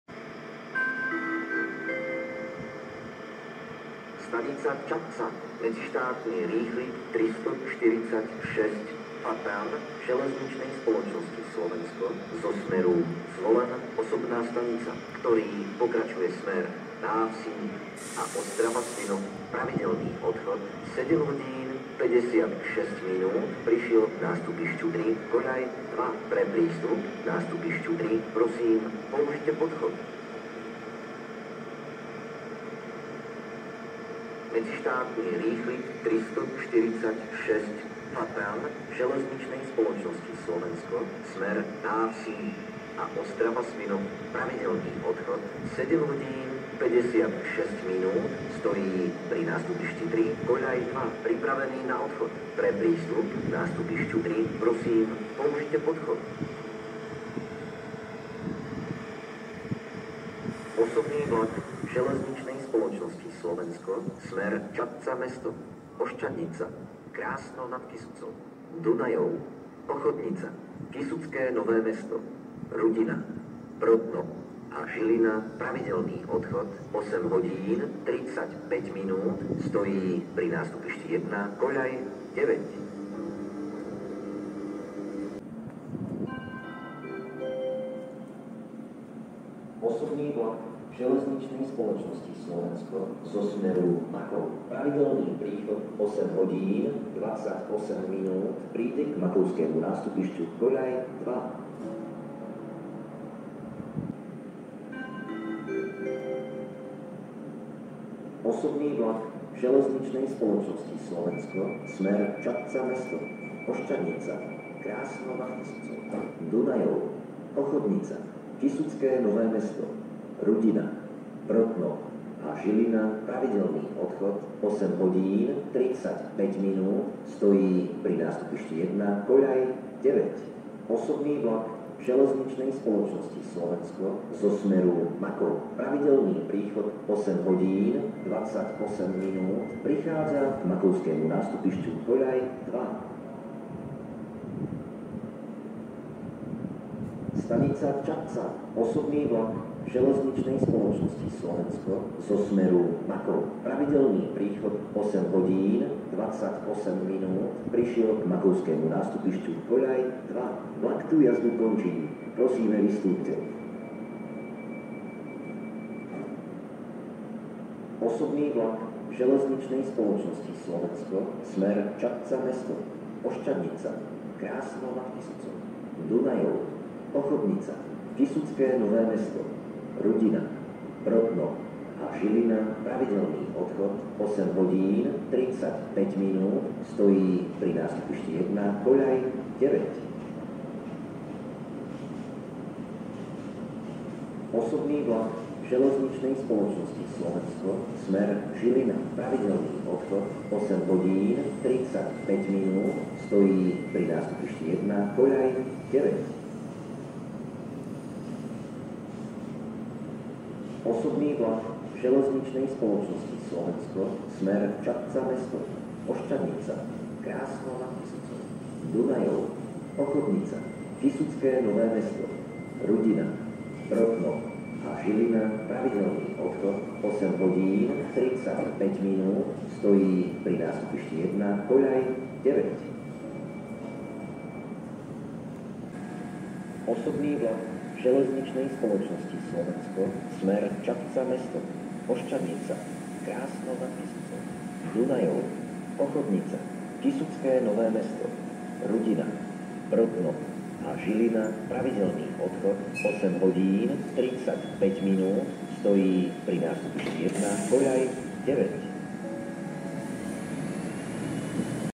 Hlášení - Čadca [SK INISS]
0:00 Mezistátní R 346 Fatran ZSSK (přijel)
0:30 Mezistátní R 346 Fatran ZSSK (ukončete nástup)
0:56 Os ZSSK (nástup)
1:26 Os ZSSK (přijede)
1:44 Os ZSSK (nástup)
2:11 Os ZSSK (přijíždí)
2:23 Os ZSSK (přijel)
2:43 Os ZSSK (nástup)
3:10 Os ZSSK (nástup)
3:26 Os ZSSK (nástup)
3:56 Os ZSSK (nástup)

Popis doplnil: Kuba69